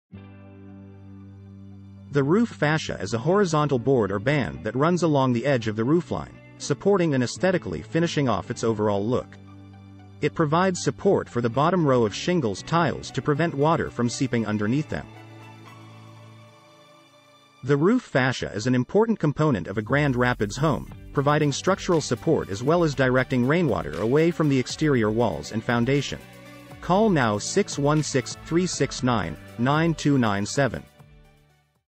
Understanding Roof Fascia: Its Definition, Importance, and Role in Roofing
The roof fascia is a horizontal board or band that runs along the edge of the roofline, supporting and aesthetically finishing off its overall look. It provides support for the bottom row of shingles/tiles to prevent water from seeping underneath them.

The roof fascia is an important component of a Grand Rapids home, providing structural support as well as directing rainwater away from the exterior walls and foundation. It helps to protect against potential damage caused by water runoff due to unpredictable weather conditions. Regular maintenance and timely repairs are essential for optimal functionality and longevity.

-The Importance of Properly Installed and Maintained Roof Fascia
-Common Issues with Roof Fascias and How to Address Them
-Choosing the Right Materials for Your Roof Fascias